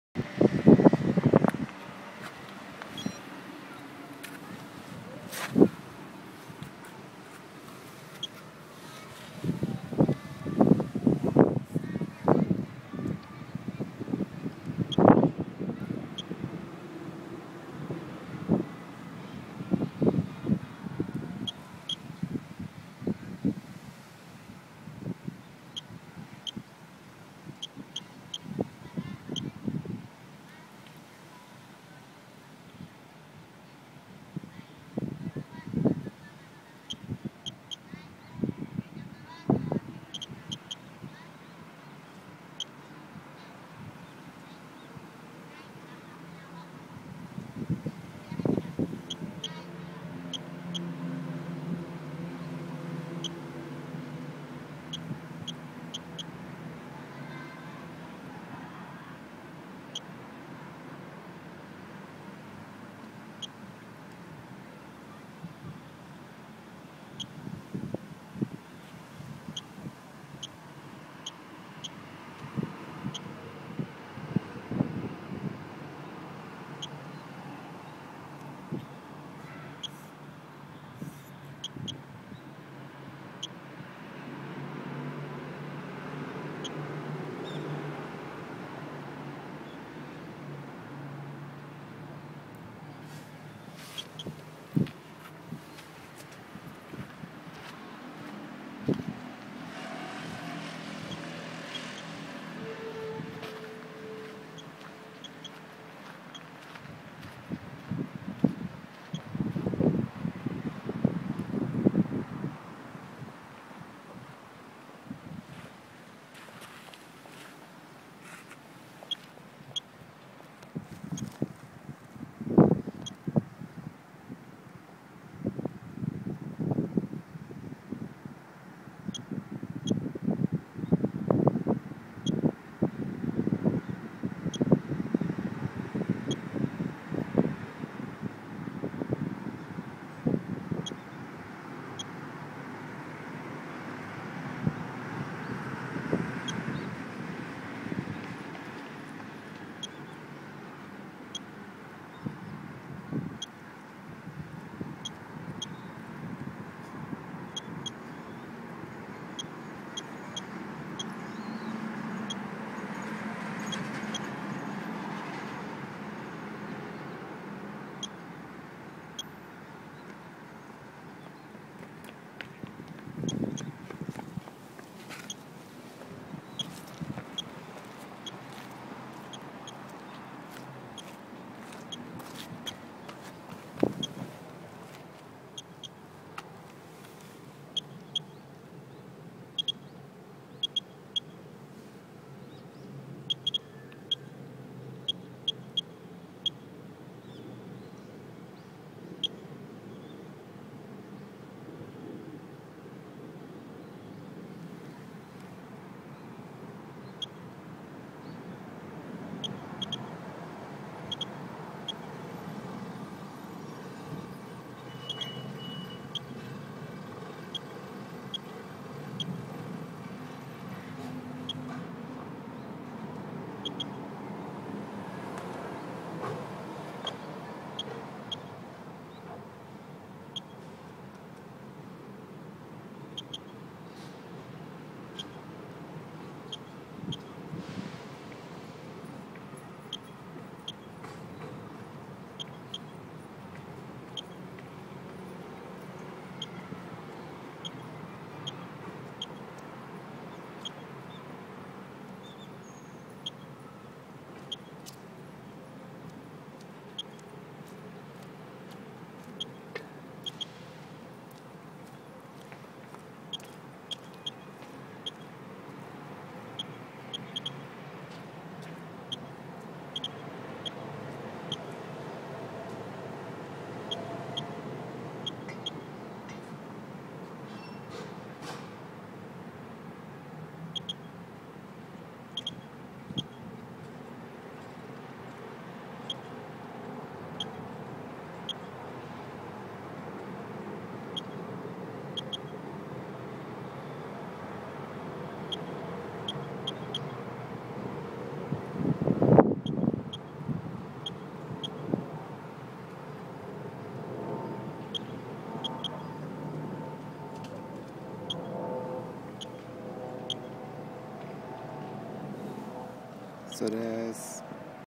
Radioactivity in Tokyo on a Playground, 12/18/2012
The Levels are not that high but the last Reading give us more than we get in Average!

When you stand there, for a whole Year and without Food you will accumulate enough to be above the Limit!